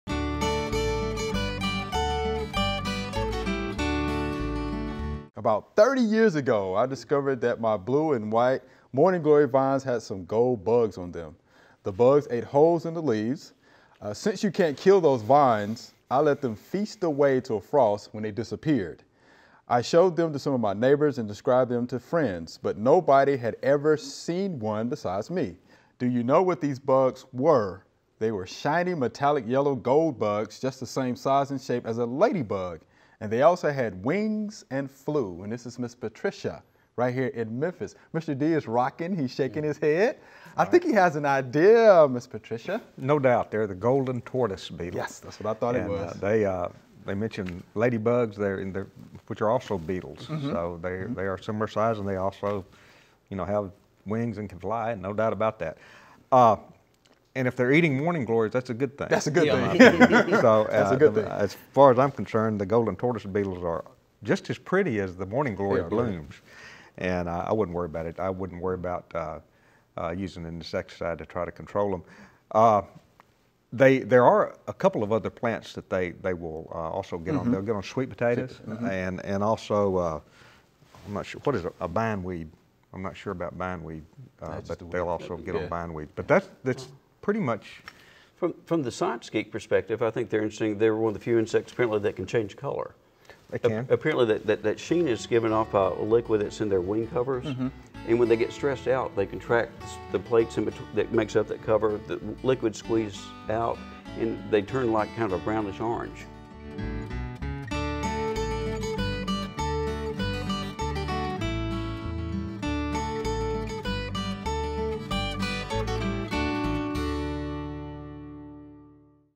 Q&A - Gold Beetle On My Morning Glory
Golden tortoise beetles feed on only three plants: morning glory, sweet potato, and bindweed. In the home garden they are mostly found on morning glory. As morning glory is such a vigorous plant it is not necessary to control the golden tortoise beetle with insecticides.